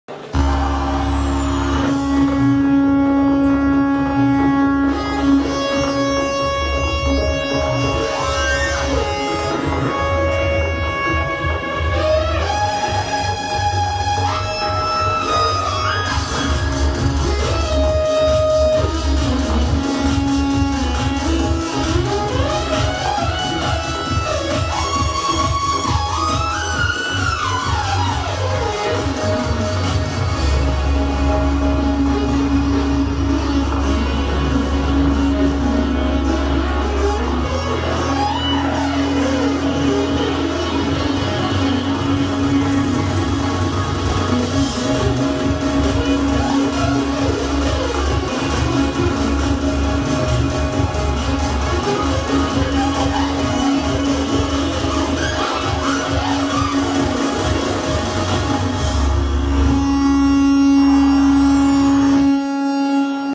Robero Mauri roland montecampione music village2009
Roberto Mauri presenta la roland juno di al music village 2009 di montecampione